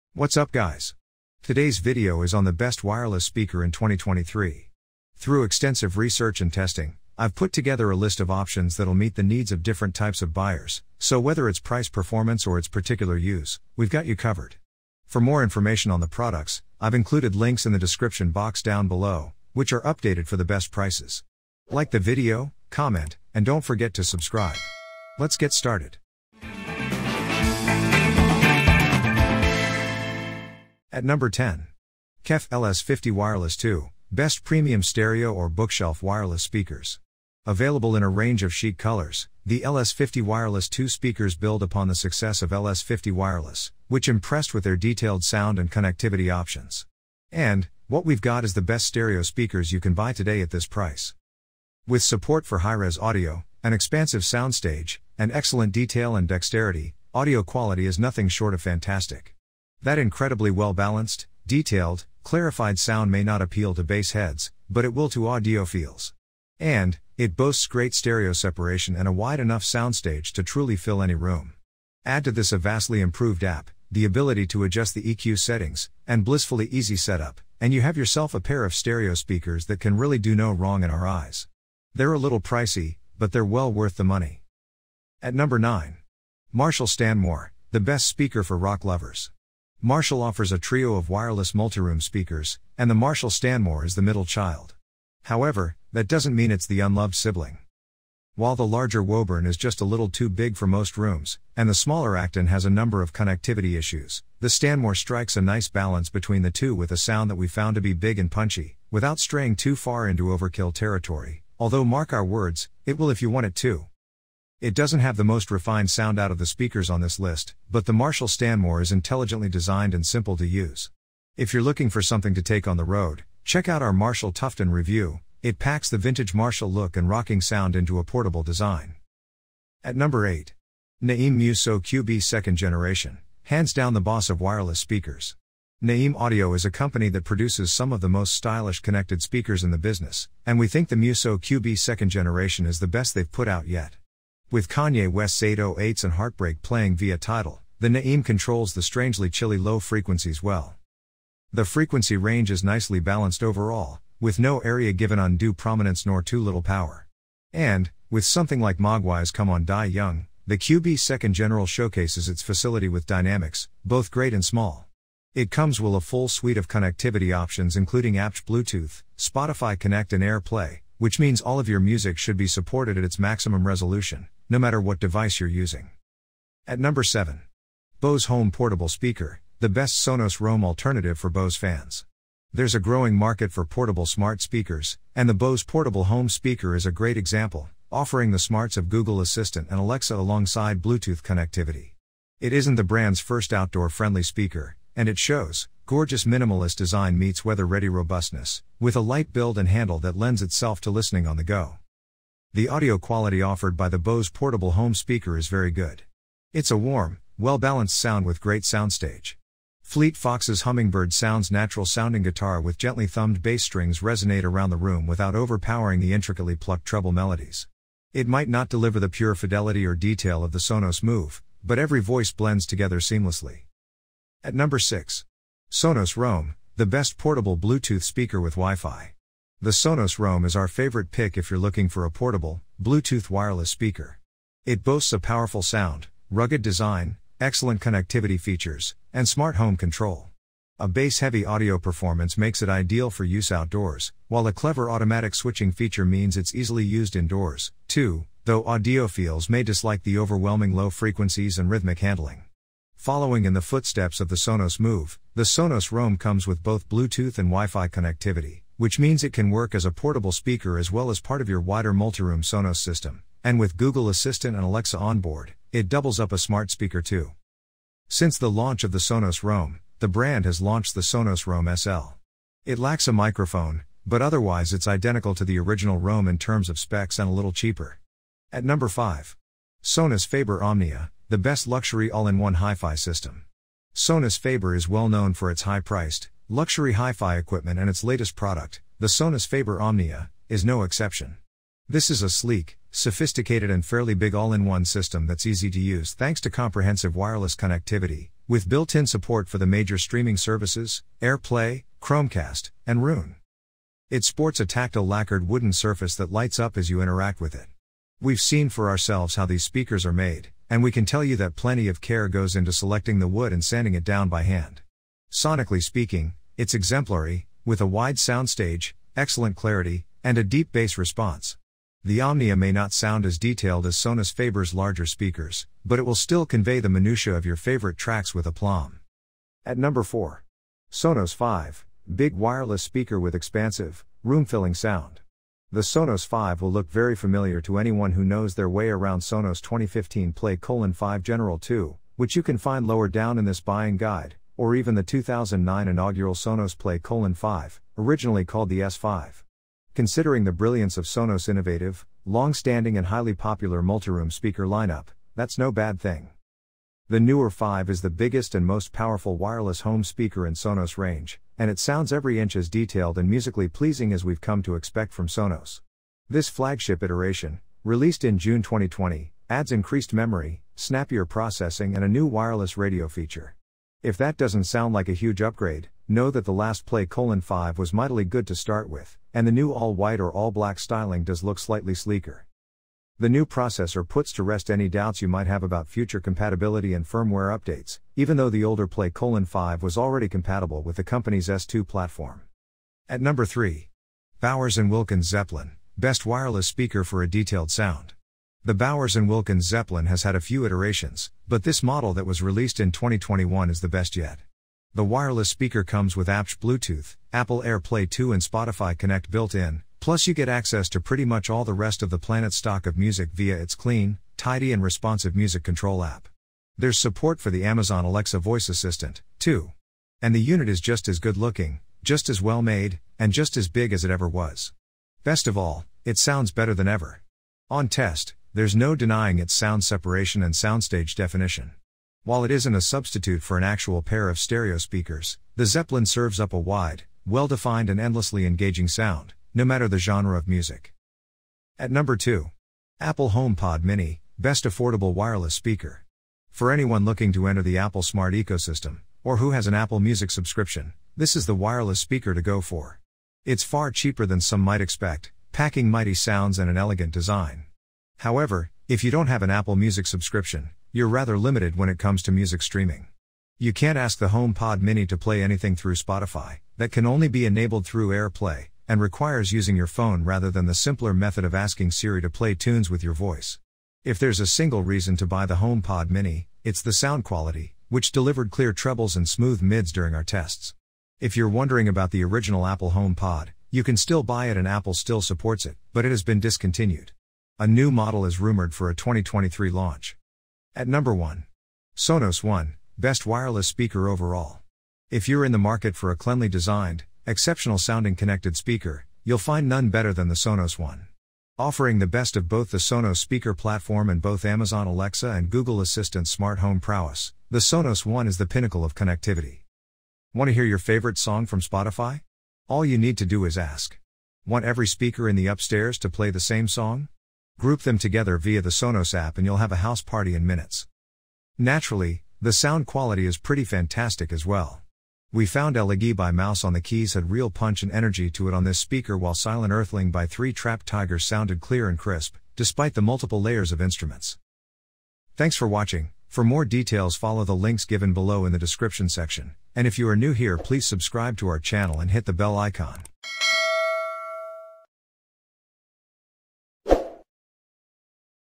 Top 10 Best Wireless Speakers for 2023 | The Ultimate Guide
Best wireless speaker for 2023
1. Sonos One: The best wireless speaker overall.

2. Apple HomePod mini: The best affordable wireless speaker if you're on a budget.

3. Bowers & Wilkins Zeppelin (2021): The best wireless speaker for a detailed sound.

4. Sonos Five: Big wireless speaker with expansive, room-filling sound.

5. Sonus Faber Omnia: The best luxury all-in-one hi-fi system.

6. Sonos Roam: The best portable Bluetooth speaker with Wi-Fi.

7. Bose Home Portable Speaker: The best Sonos Roam-alternative for Bose fans.

8. Naim Mu-So Qb 2nd Generation: Hands-down the boss of wireless speakers.

9. Marshall Stanmore: The best speaker for rock lovers.

10. KEF LS50 Wireless II: The best premium stereo or bookshelf wireless speakers.

Welcome to our expert review and buying guide of the Top 10 Best Wireless Speakers for 2023. In this video, we've handpicked the best wireless speakers that you can buy in 2023, based on their sound quality, design, features, and value for money.

Sonos One: The best wireless speaker overall - Enjoy rich, room-filling sound with deep bass and crystal-clear highs. With voice control, you can easily manage your music and smart home devices with Amazon Alexa and Google Assistant.
Apple HomePod mini: The best affordable wireless speaker if you're on a budget - It offers a powerful, room-filling sound and integrates seamlessly with your Apple devices. With Siri, you can control your music, set timers, and get news and weather updates.
Bowers & Wilkins Zeppelin (2021): The best wireless speaker for a detailed sound - This speaker delivers incredibly detailed and accurate sound with its unique acoustic design. With its stunning looks, it will enhance the aesthetics of any room.
Sonos Five: Big wireless speaker with expansive, room-filling sound - This speaker is perfect for large rooms and outdoor spaces. It offers an immersive, three-dimensional soundstage that lets you hear every detail in your music.
Sonus Faber Omnia: The best luxury all-in-one hi-fi system - It's a perfect blend of style and substance. With its premium materials, beautiful design, and exceptional sound quality, it's the ultimate luxury wireless speaker.
Sonos Roam: The best portable Bluetooth speaker with Wi-Fi - It's a portable, lightweight speaker that you can take anywhere. It offers excellent sound quality, voice control, and automatic Wi-Fi switching.
Bose Home Portable Speaker: The best Sonos Roam-alternative for Bose fans - It delivers powerful, room-filling sound with deep bass and clear highs. With Alexa and Google Assistant, you can control your music and smart home devices with ease.
Naim Mu-So Qb 2nd Generation: Hands-down the boss of wireless speakers - It's a high-end wireless speaker that delivers incredible sound quality, exceptional design, and advanced features. It's perfect for audiophiles who demand the best.
Marshall Stanmore: The best speaker for rock lovers - It delivers a classic rock sound with its powerful bass and vintage looks. With its Bluetooth and wired connectivity, you can enjoy your music in any format.
KEF LS50 Wireless II: The best premium stereo or bookshelf wireless speakers - These speakers offer exceptional sound quality, stunning design, and advanced features. They're perfect for music lovers who want the best sound possible.
So, whether you're looking for a wireless speaker for your home, office, or outdoor activities, we've got you covered.